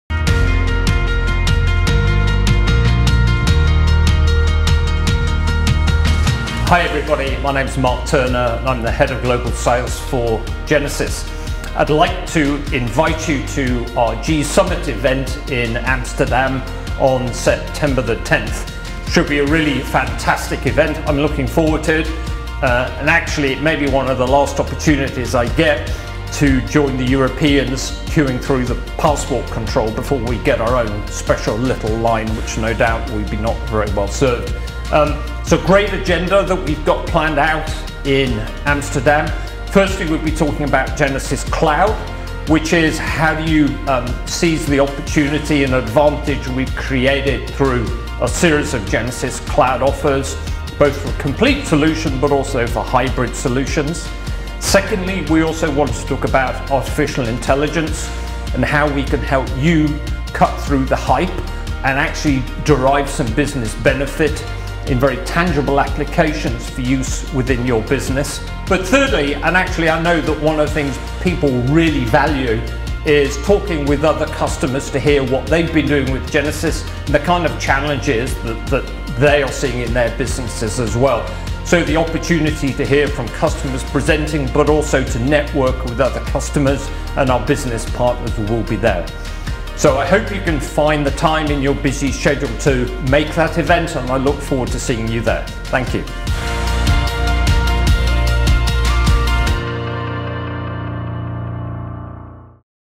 Mark Turner's Invitation to G-Summit Europe
Mark Turner Genesys EVP, Global Sales & Field Operations extends a personal invitation to join him and the Genesys community of customers and partners at Europe's definitive CX event of the year, G-Summit Europe in Amsterdam 10–12 September 2019.

You'll learn about the opportunities that Genesys Cloud presents, you'll explore how to realize business benefits with AI and finally, you'll be able to do the valuable networking with other customers about how they're using Genesys solutions to deliver the best possible experience to their customers.